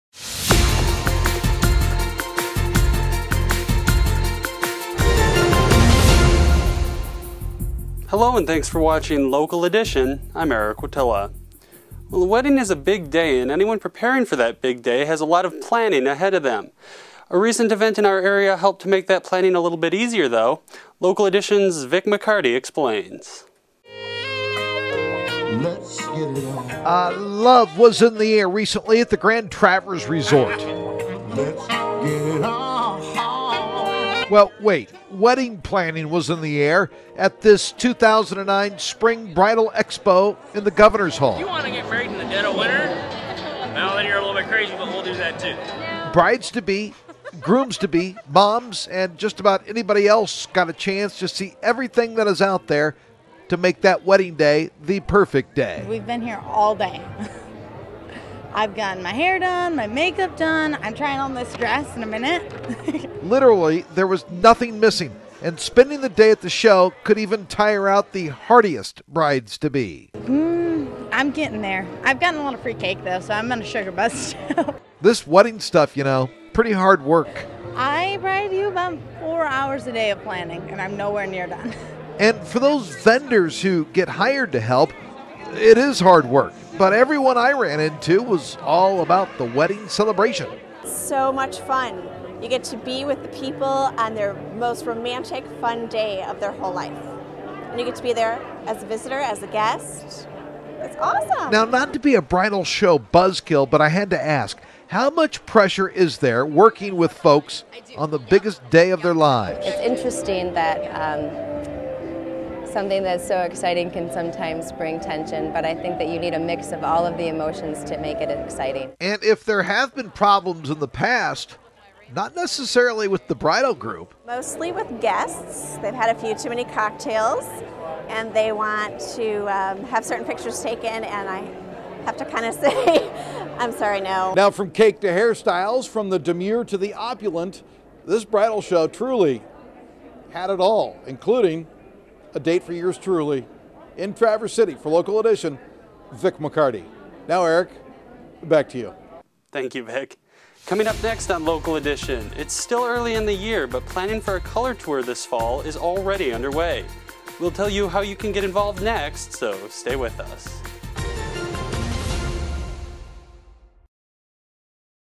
Wedding Expo at Grand Traverse Resort
A wedding is a big day, and anyone preparing for that big day has a lot of planning ahead of them. A recent event in our area helped to make that planning a little easier, though; Vic McCarty Explains.